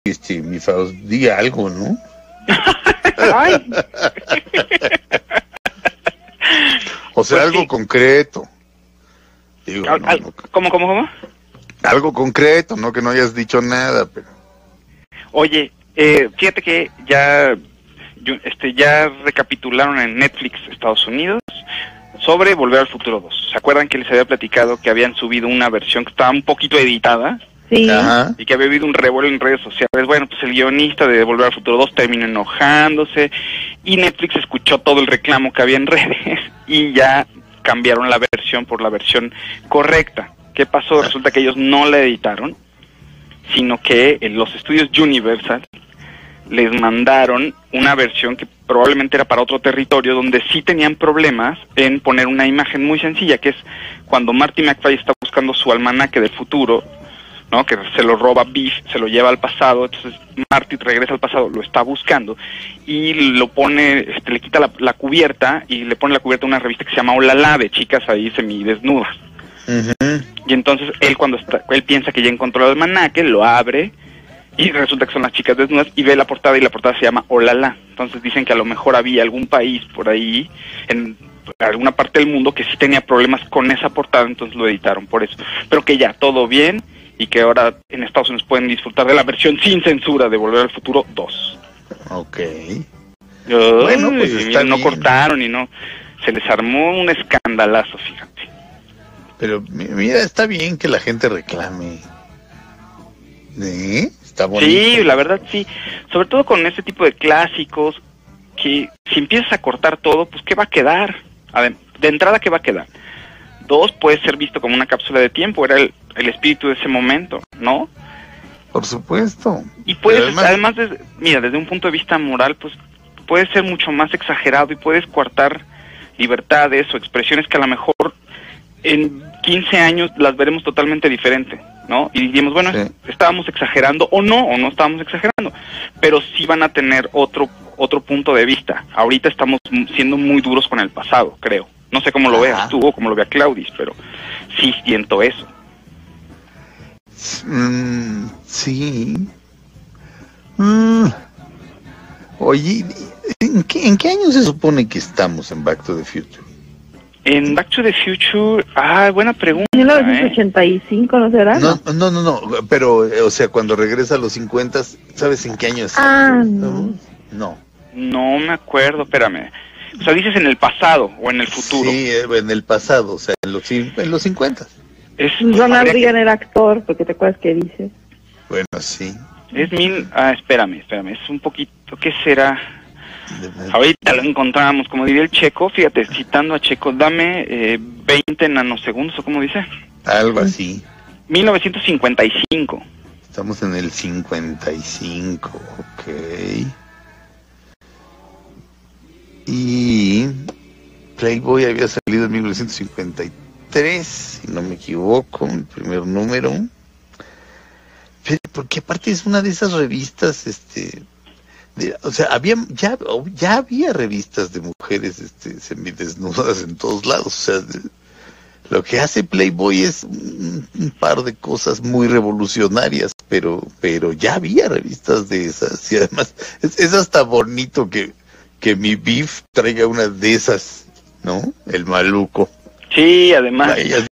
Netflix no censuró la escena de Volver al Futuro 2.
Netflix no censuró 'Regreso al futuro II': el guionista de la mítica trilogía explica que fue un problema de Universal.